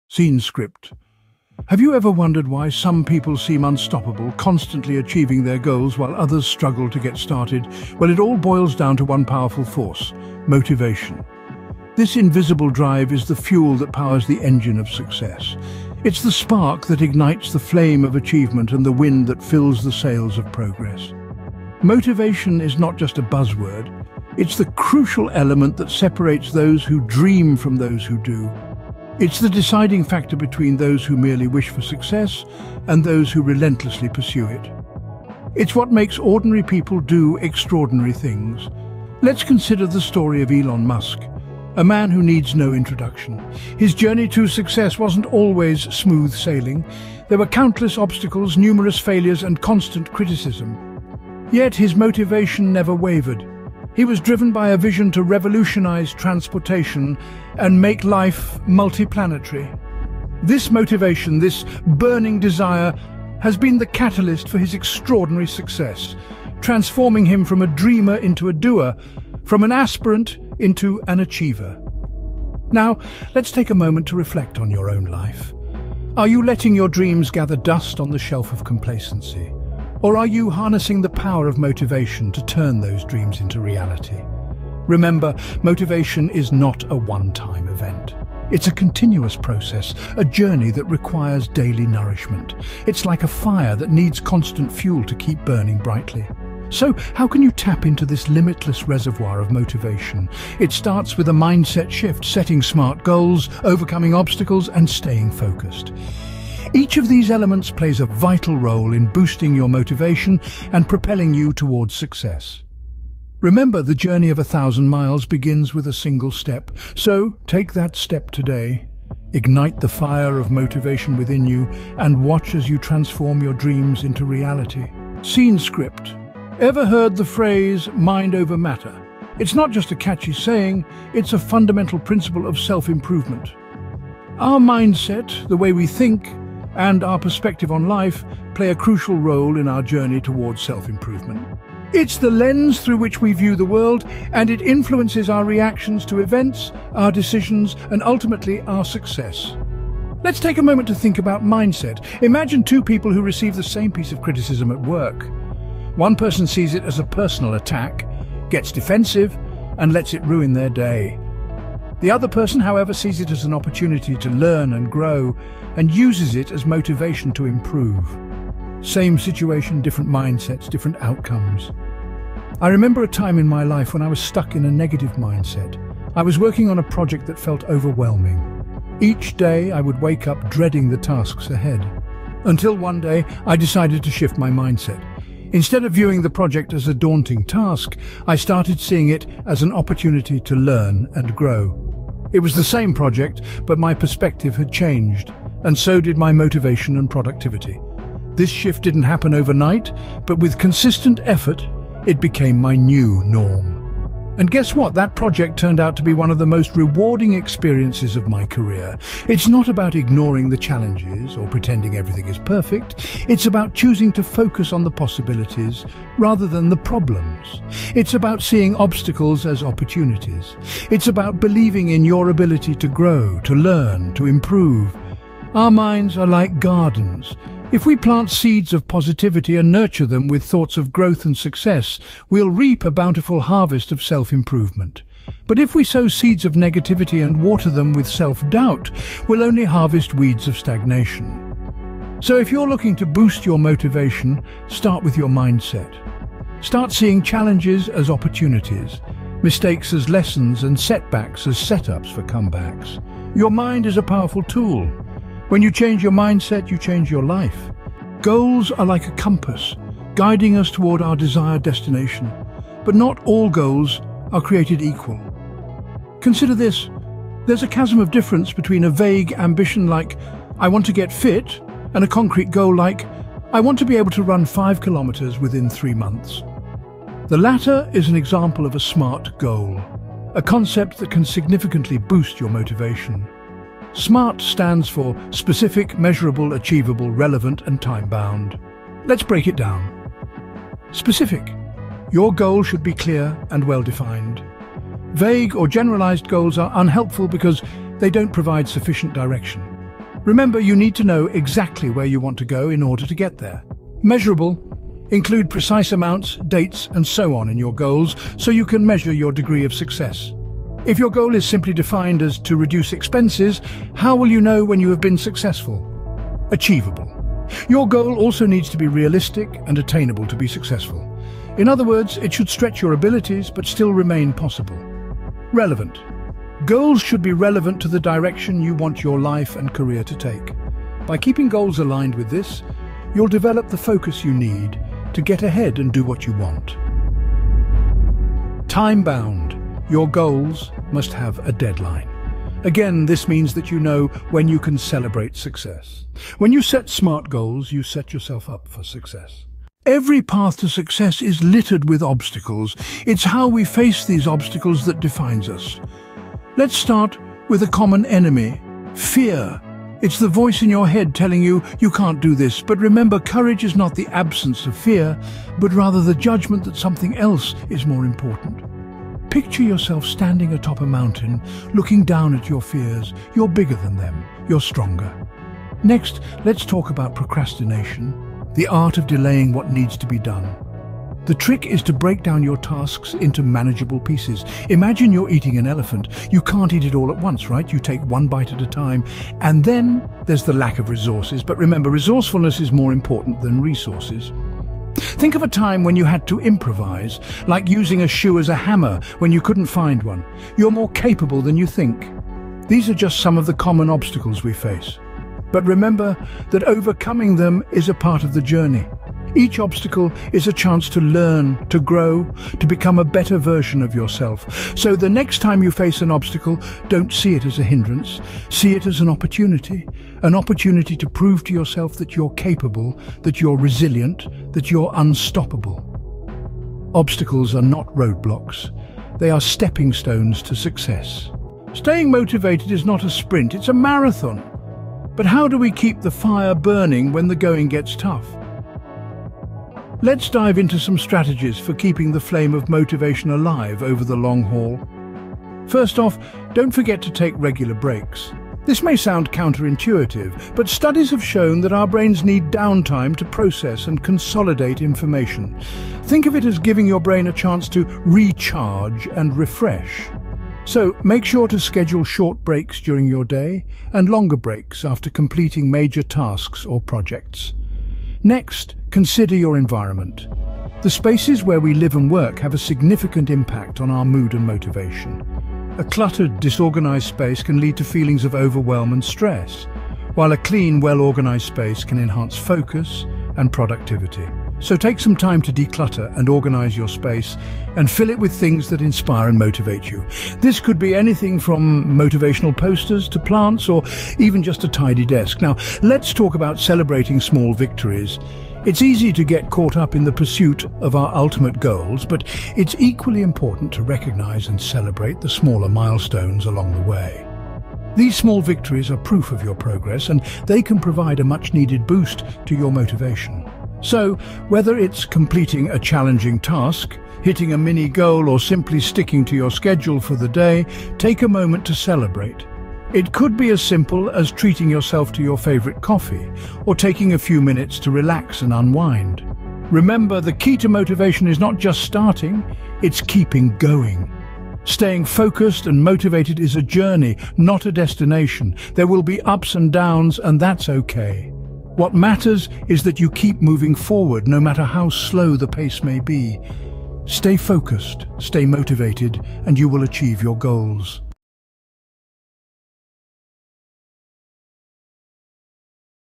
Boost Your Motivation: A Journey to Self-Improvement and Success
Fuel your fire with our empowering video on motivation. Explore strategies to boost motivation, set meaningful goals, and overcome obstacles for success.